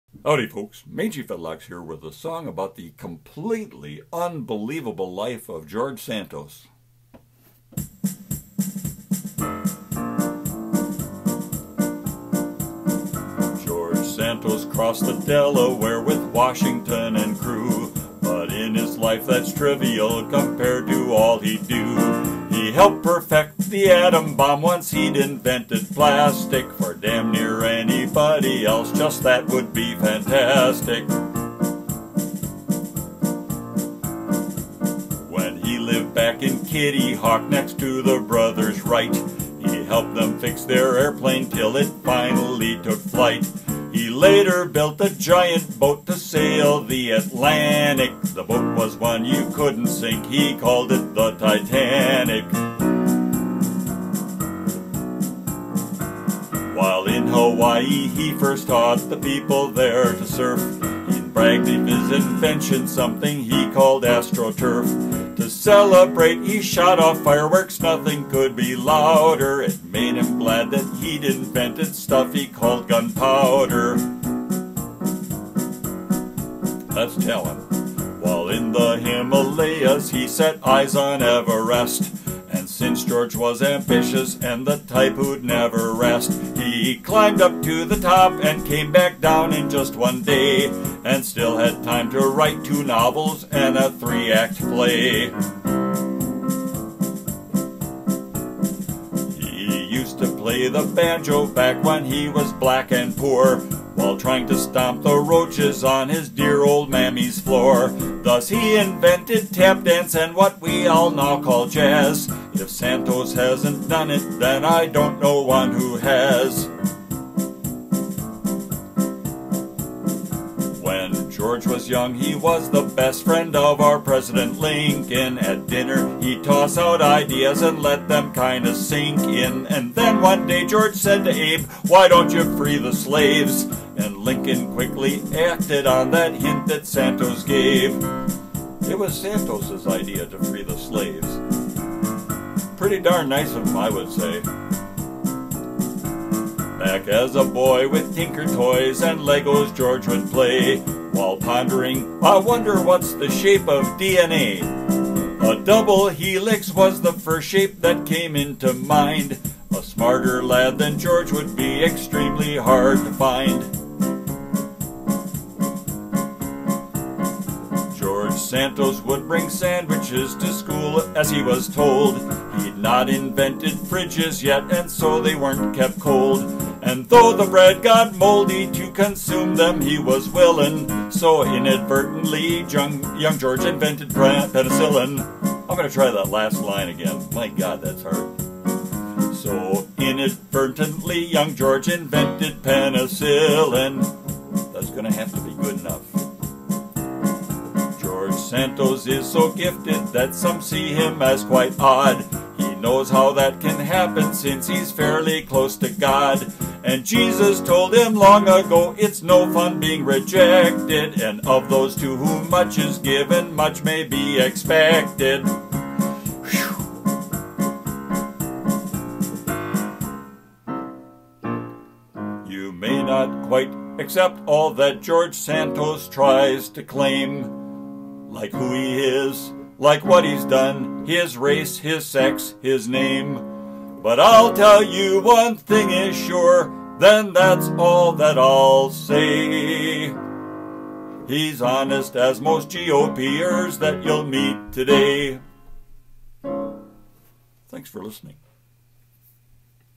The Totally Unbelievable Life of George Santos
George Santos crossed the Delaware with Washington and crew
but in his life, that’s trivial compared with all he’d do.
He helped perfect the atom bomb once he’d invented plastic.
For damn near anybody else, just THAT would be fantastic.

When he lived back in Kitty Hawk, next to the brothers Wright
he helped them fix their airplane ’til it finally took flight.
He later built a giant boat to sail the Atlan’ic
the boat was one you couldn’t sink.  He called it The Titanic.

While in Hawaii, he first taught the people there to surf,
and bragged of his invention, something he called “Astroturf.”
To celebrate, he shot off fireworks. Nothing could be louder
It made him glad that he’d invented stuff he called ‘gunpowder.’

While in the Himalayas, he set eyes on Everest,
and since George was ambitious and the type who’d never rest
he climbed up to the top, and came back down in just one day
and still had time to write two novels and a three-act play.

He used to play the banjo back when he was black and poor
while trying to stomp the roaches on his dear old mammy’s floor.
Thus, he invented tap dance and what we all now call “jazz”.
If Santos hasn’t done it, then I don’t know one who has.

When George was young he was the best friend our President Lincoln
At dinner, he’d toss out ideas and let them kind of sink in.
and then one day, George said to Abe,  “Why don’t you free the slaves?”
and Lincoln quickly acted on that hint that Santos gave.

Back as a boy with Tinker Toys and Legos George would play
while pondering “I wonder that’s the shape of DNA?”
A double-helix was the first shape that came into mind.
A smarter lad than George would be extremely hard to find.

George Santos would bring sandwiches to school as he was told
He’d not invented fridges yet, so they weren’t kept cold.
and though the bread got moldy, to consume them he was willin’
so inadvertently young George invented penicillin.

George Santos is so gifted that some see him as quite odd.
He knows how that can happen, since he’s fairly close to God
and Jesus told him long ago, it’s no fun being rejected
but of those to whom much is given, much may be expected.

You may not quite accept all that George Santos tries to claim
like who he is, like what he’s done, his race, his sex, his name,
but I’ll tell you, one thing is sure, then that’s all that I’ll say...
he’s honest as most GOPers that you’ll meet today.